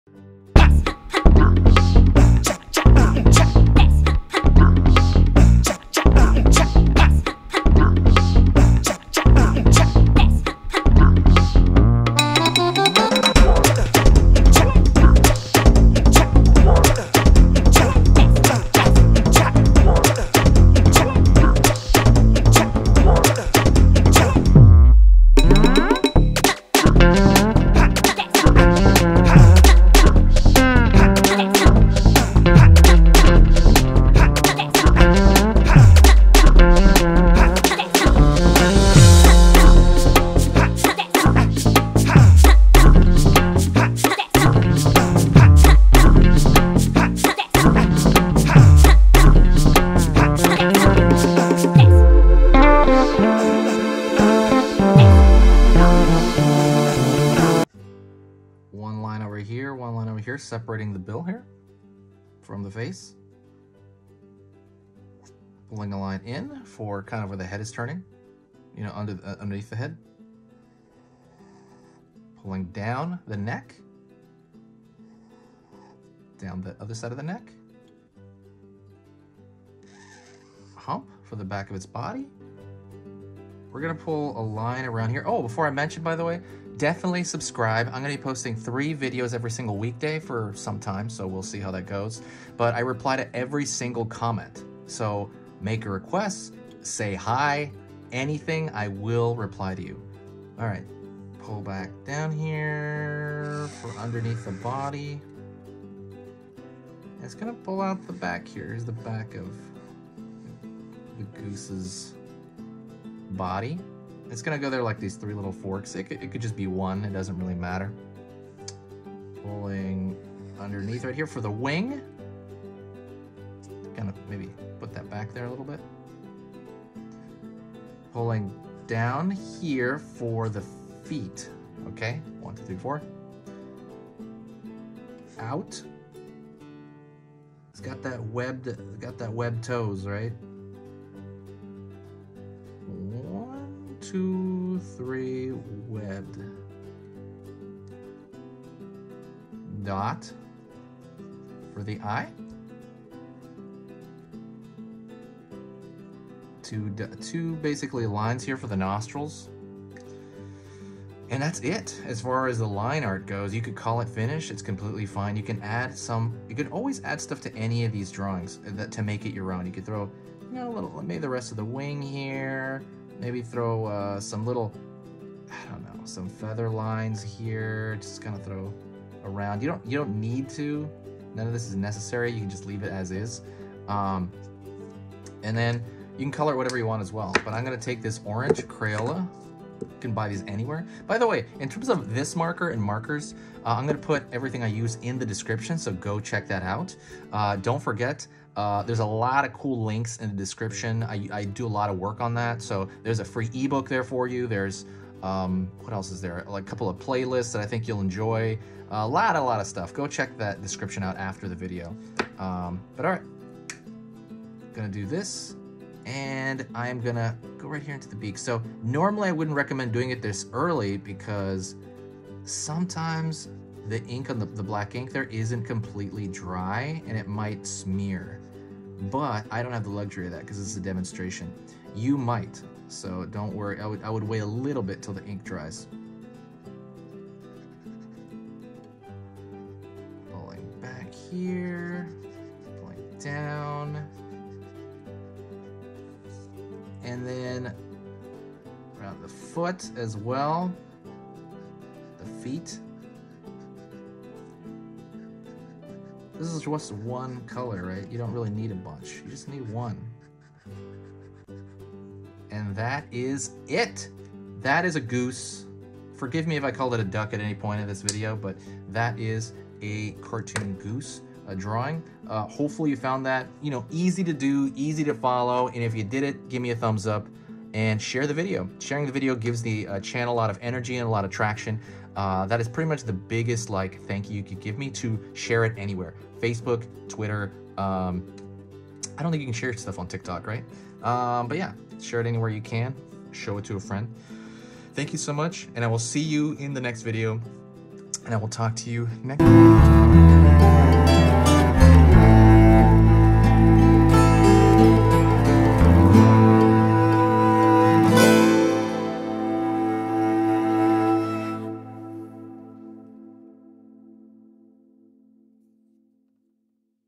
How to Draw a Goose - Cute Animals- Easy Pictures to Draw
OR

OR

In this drawing lesson, I’m going to go over how to draw a goose. A goose is really fun and easy to draw. This is meant to be done step-by-step for complete beginners. If you love to draw, you’re going to love this. This is very simple and very easy. The entire point of this lesson is for it to be extremely easy to do along with me. So I really hope you have an amazing time drawing this goes with me. Cute animals are always fun to draw. This leaves a little bit realistic, but more on the cartoonish side. I hope you have an amazing day and you come along with me on this exciting journey. One of the coolest things about drawing is learning together. And I’m hoping that I Teach you many cool things that you can use in the future. Thank you so much and let’s get started on this cool drawing lesson. One more thing if you enjoy the video please share it. There’s a lot of cool things and I’d love for you to see them. Thank you so much and let’s get started.

Cool Links __________________________________________________